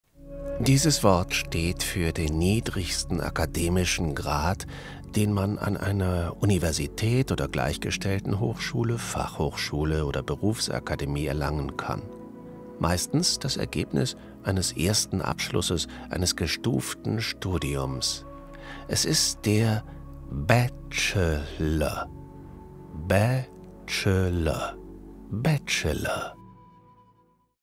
Bachelor. Richtige Sprechweise, Bedeutung. Korrekte Aussprache. Gutes Deutsch. Von TV-Profisprecher.
Bachelor. Wie spricht man das? Was ist das?. Gutes Deutsch. Ich helfe bei Zweifelsfällen der deutschen Aussprache.
Bachelor (Tasse)
Bachelor (T-Shirt)
Korrektes wissenschaftliches Arbeiten
Zeitmanagement im Studium
Wichtige Wirtschaftsgesetze für Bachelor/Master

Das Standardwerk für die deutsche Aussprache